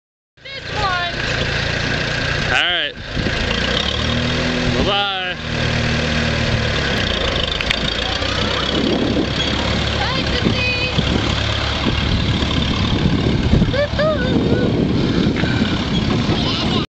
Cruising on the 20
The quietest smoothest ridin tractor we have so far